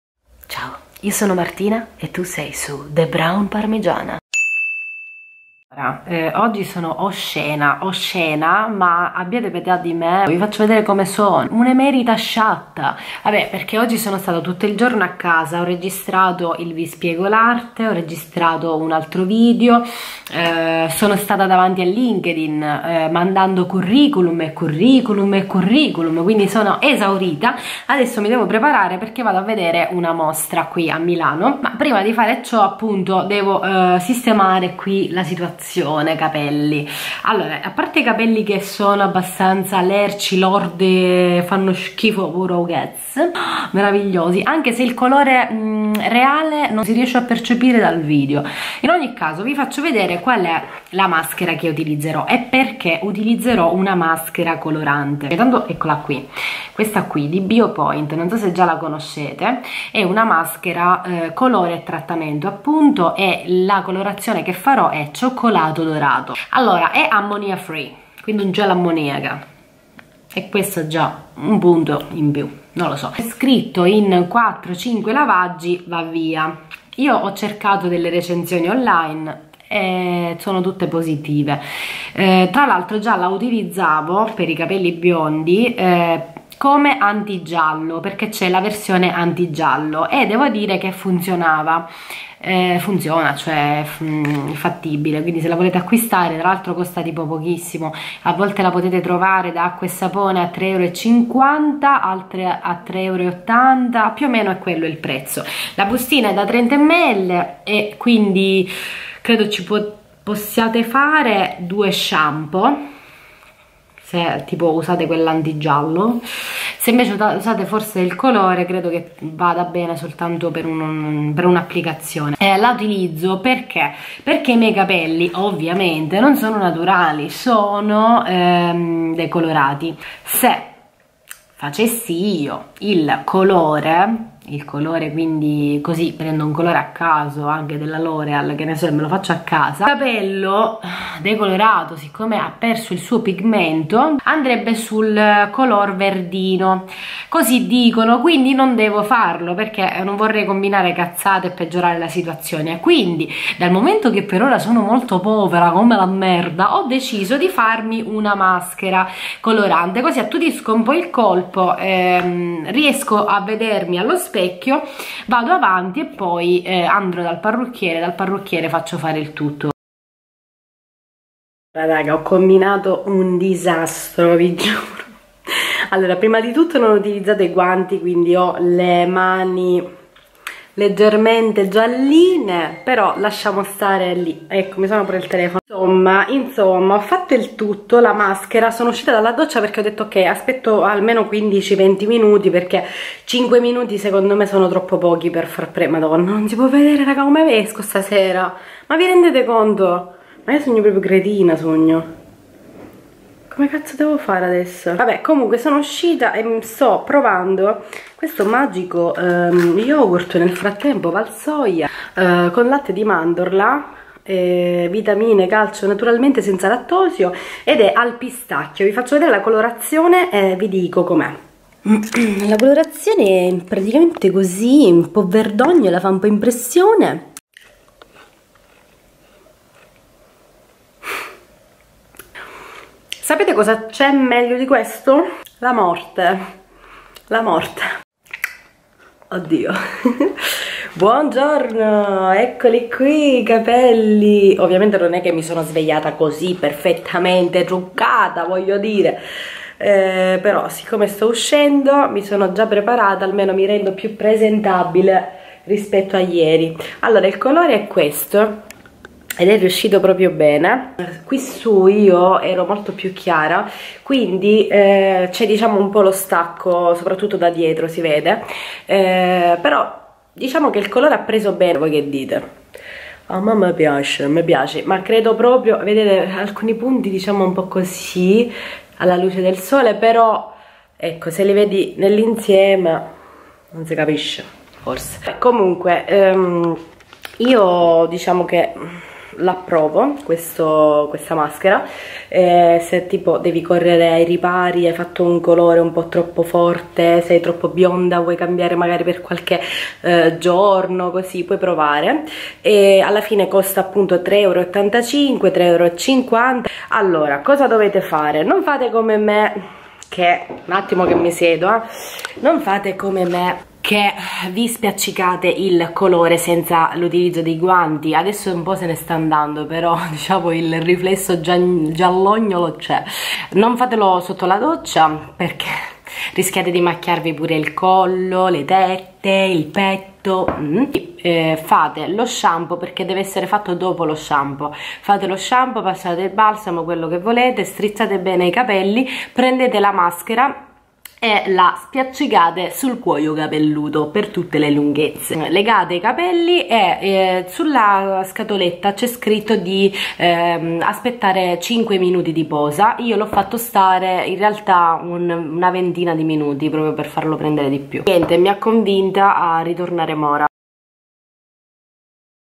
DA BIONDA 👩🏼 A MORA 👩🏻 SENZA TINTA 👌🏻- fai da te
CIAO! in questo video, presa dalla disperazione, ti faccio vedere come ho cambiato colore dei capelli semplicemente a casa, con la maschera della Biopoint color mask.

Se hai dubbi e domande scrivimi nei commenti!